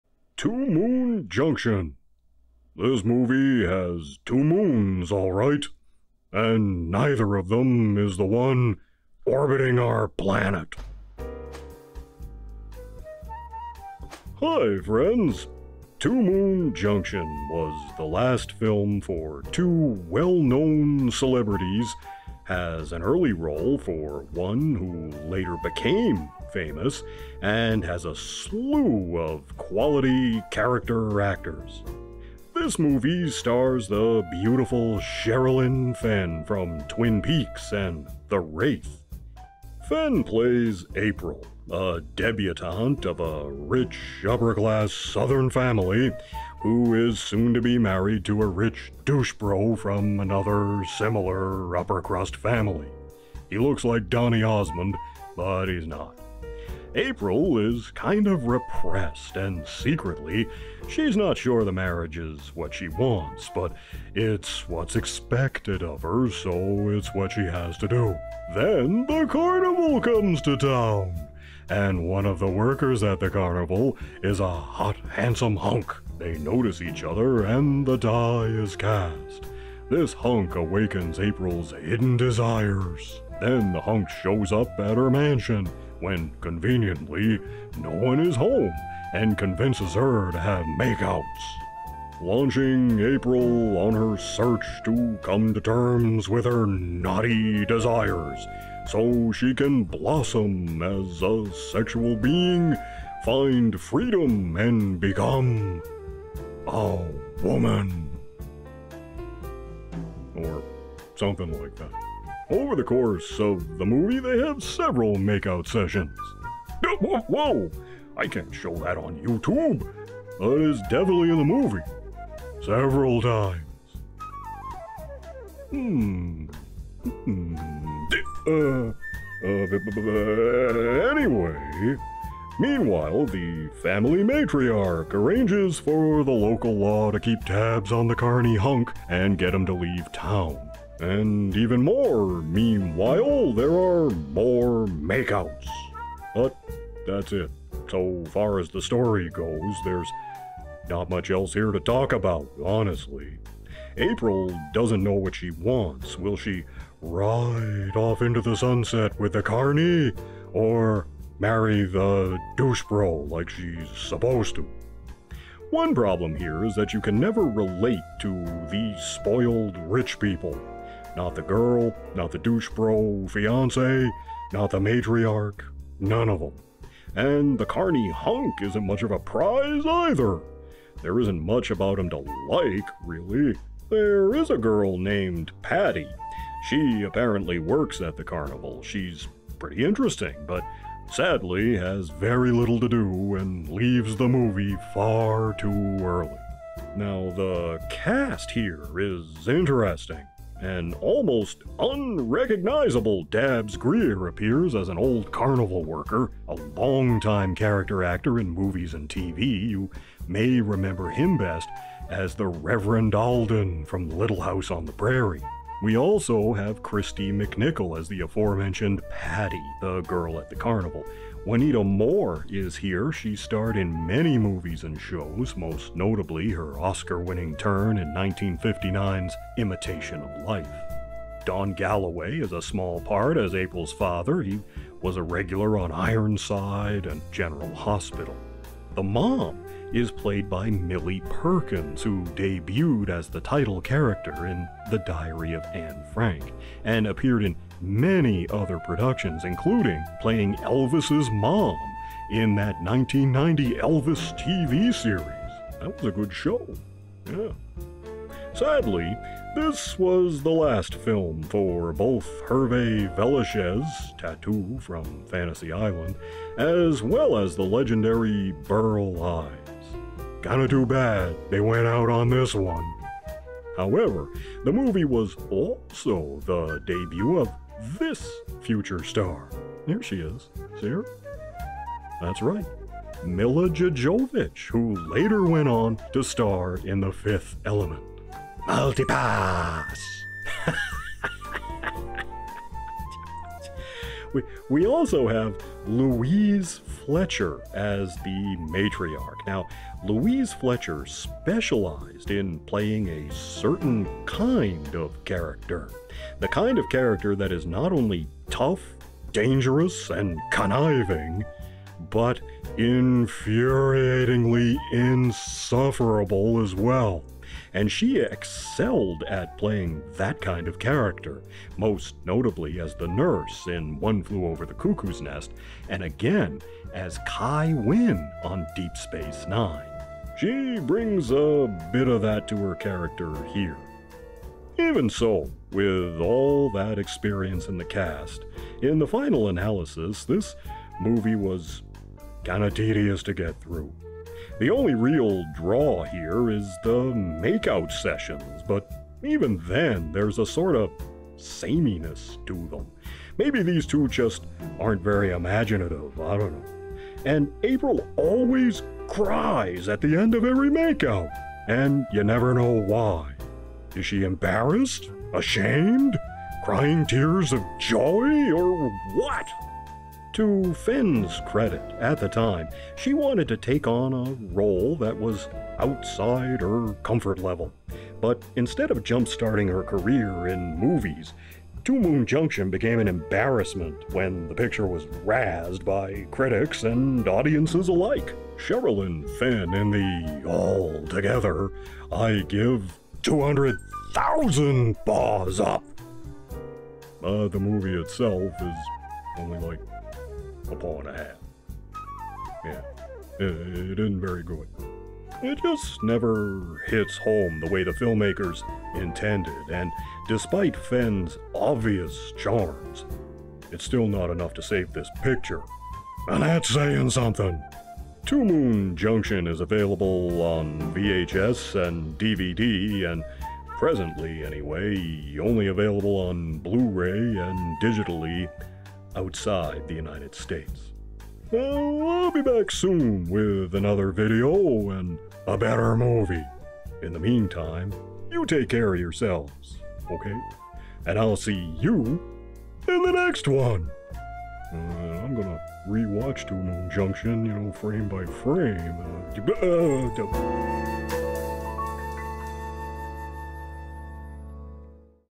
"Two Moon Junction" (1988) Movie Review
This time Barkey reviews a movie from 1988 that has several interesting elements, including the final role for two actors, and the debut of a future star. "Two Moon Junction" (1988) is the story of a repressed young upper class woman who wants to walk on the wild side, and a traveling carnival provides the answer in the form of a handsome hunk carny. Sherilyn Fenn and several notable character actors star.

"Two Moon Junction" (1988) is available on VHS and DVD.

Barkey's Wish List